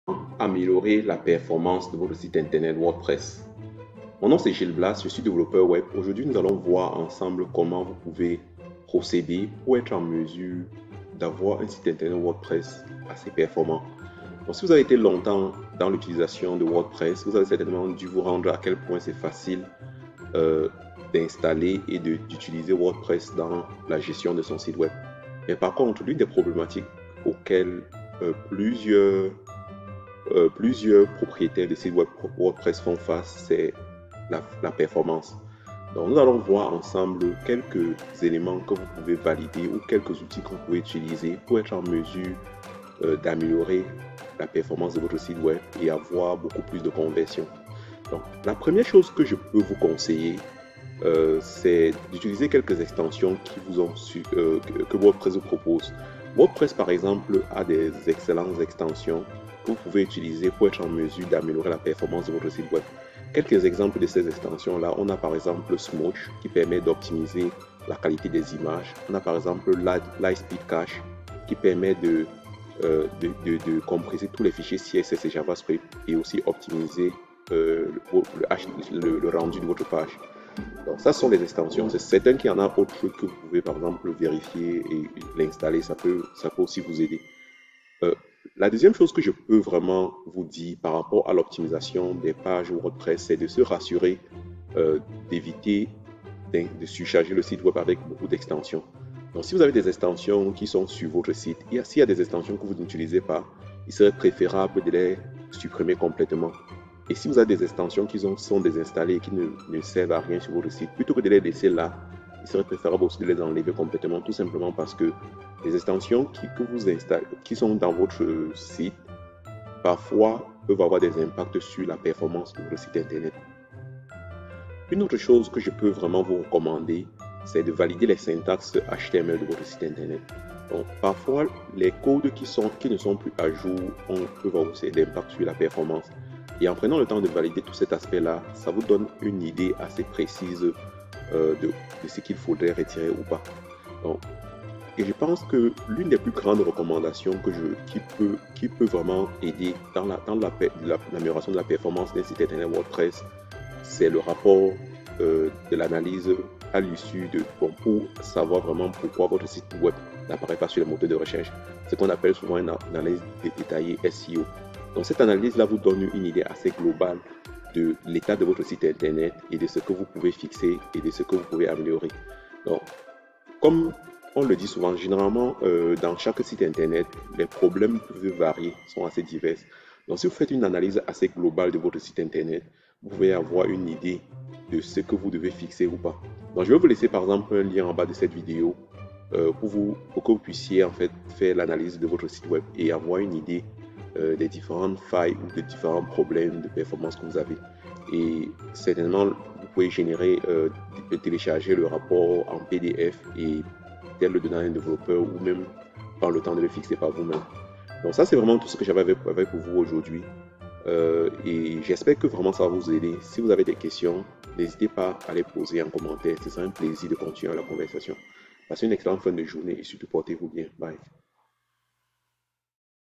Comment améliorer la performance de son site web WordPress
Comment améliorer la performance de site web WordPress ?  Quatre outils qui peuvent vous aider.
• Temps de chargement des Pages (Smush, LiteSpeed Cache)
• La performance du serveur (La configuration de votre fichier .htaccess)
• Éviter trop d’extension (supprimer complètement les extensions que vous n’utiliser pas)
• Valider la syntaxe HTML et fixer les bugs)
• Analyser votre site web et suivez les recommandations de votre analyse.